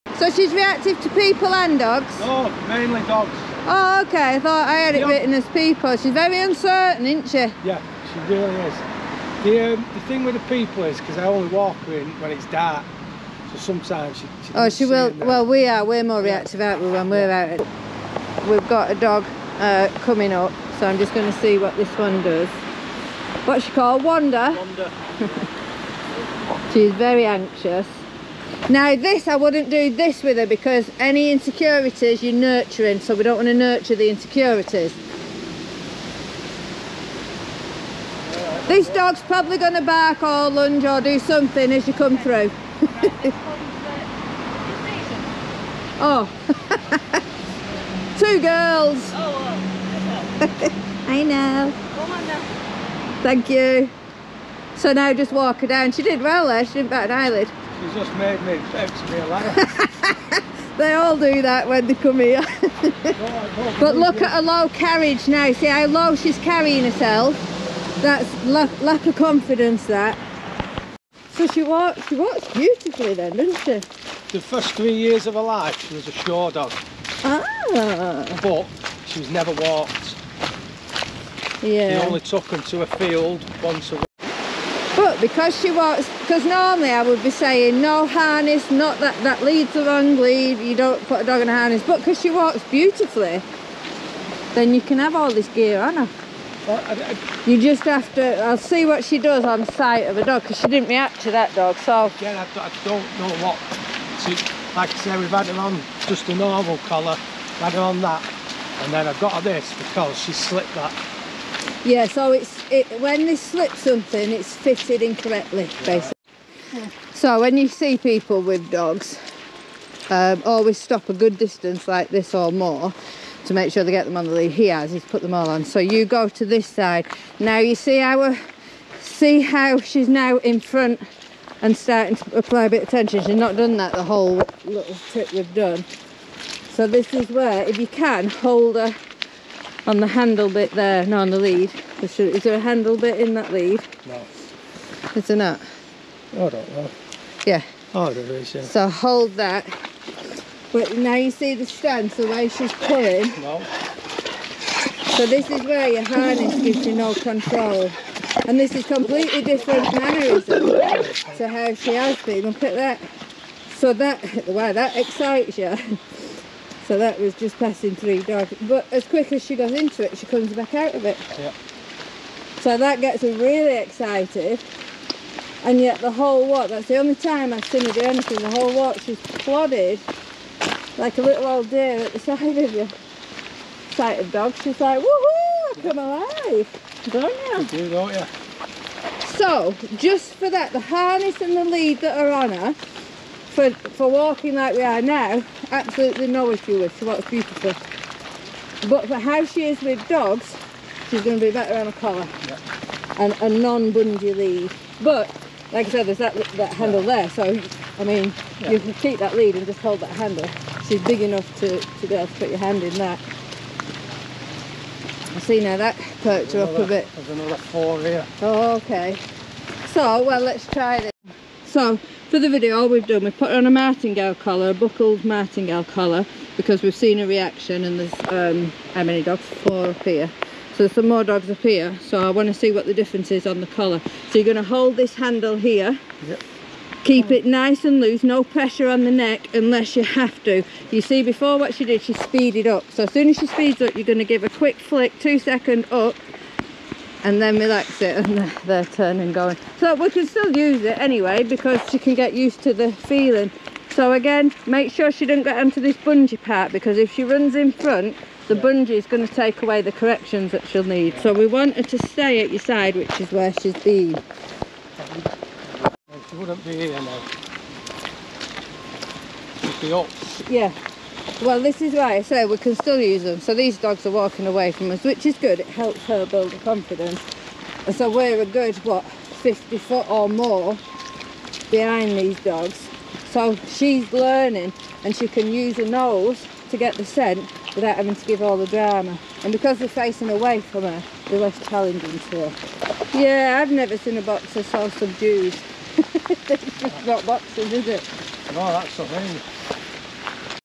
dog Reactive Boxer dog, Wanda
Wanda is an ex show/breeding dog, who unfortunately didn't love a normal dogs life, which means we have a lot of work to do with her, firstly getting her to focus on something (apart from dogs) so I've suggested to her owner that instead of feeding her from a bowl, to hand feed her while on walks, we need something to entice her with..

She very subdued, very unlike any normal boxer.
But I'm sure we can get her where she can walk past other dogs without giving all the drama.

Watch this space, she's in each week.